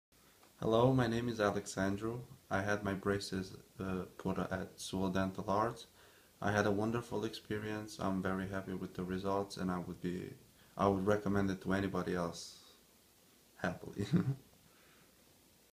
Teen Braces Video Testimonial - Dentist in Sewell / Washington Township NJ
Video testimonial about dental braces for kids and teens provided by Dr. Pastiu - Cosmetic Dentist in Sewell, NJ / Washington Township New Jersey